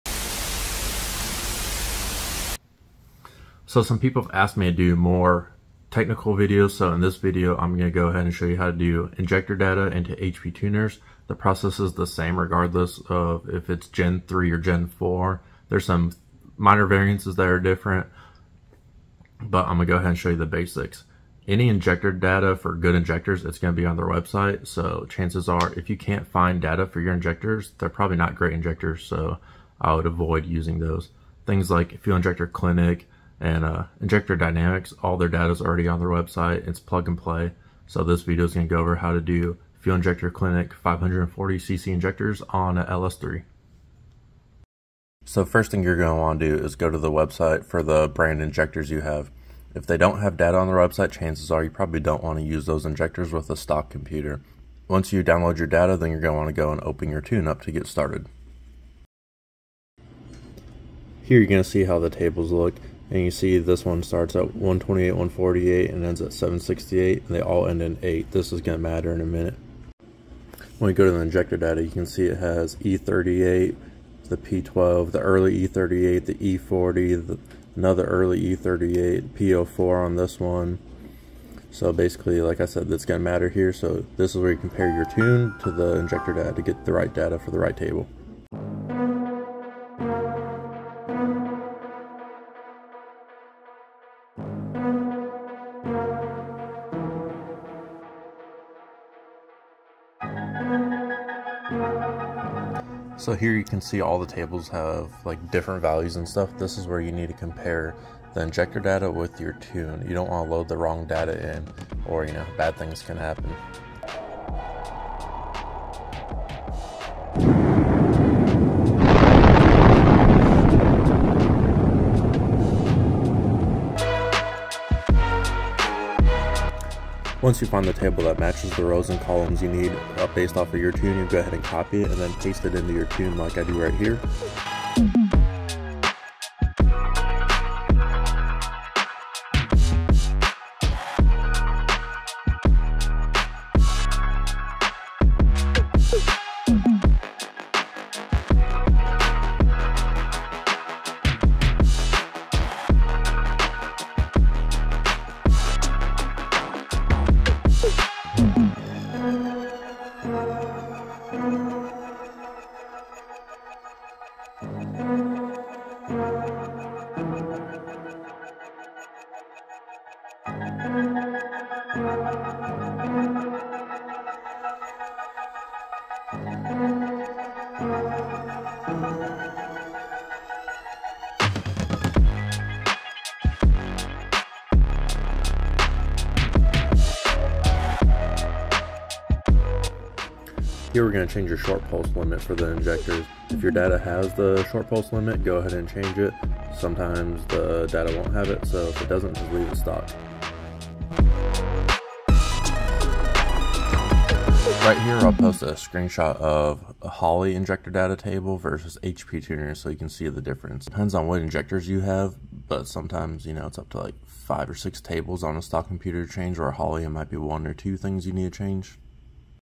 HP Tuners Step by Step Guide To Change Injector Data
Changing LS Injector Data with HP Tuners Step by Step Guide. In this video, I use a 2017 Ls3 e38 with hptuners for the examples. All data used can be found on the fuel injector clinic's website.

Tuning information